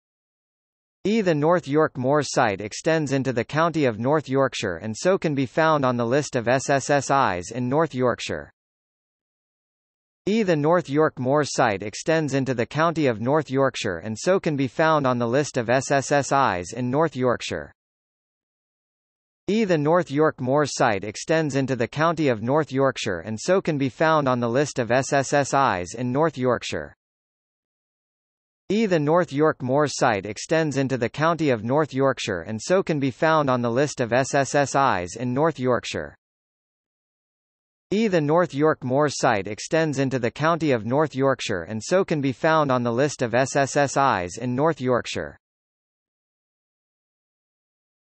List of Sites of Special Scientific Interest in Cleveland | Wikipedia audio article
00:00:18 Sites
00:00:27 Notes

- Socrates

SUMMARY
This is a list of the Sites of Special Scientific Interest (SSSIs) in Cleveland, England, United Kingdom. In England the body responsible for designating SSSIs is Natural England, which chooses a site because of its fauna, flora, geological or physiographical features. As of 2006, there are 18 sites designated within this Area of Search, of which 12 have been designated due to their biological interest, 4 due to their geological interest, and 2 (Durham Coast and Redcar Rocks) for both.Natural England took over the role of designating and managing SSSIs from English Nature in October 2006 when it was formed from the amalgamation of English Nature, parts of the Countryside Agency and the Rural Development Service. Natural England, like its predecessor, uses the 1974–1996 county system and as such the same approach is followed here, rather than splitting these sites between the County Durham and North Yorkshire lists. The data in the table is taken from Natural England's website in the form of citation sheets for each SSSI.For other counties, see List of SSSIs by Area of Search.